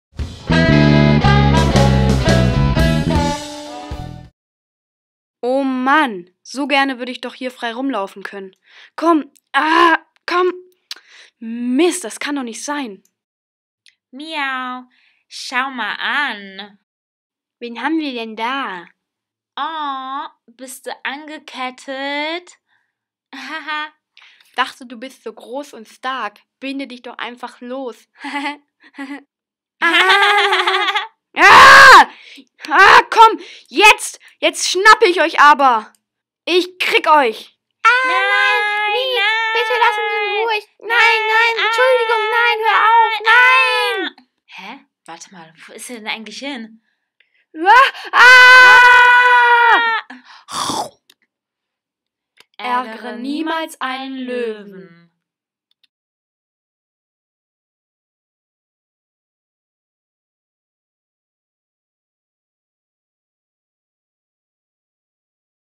Ärgere niemals einen Löwen
Gabi (FSJ Kultur) und Luis (Schülerpraktikum) produzierten einen Trickfilm zu einer selbst verfassten Fabel.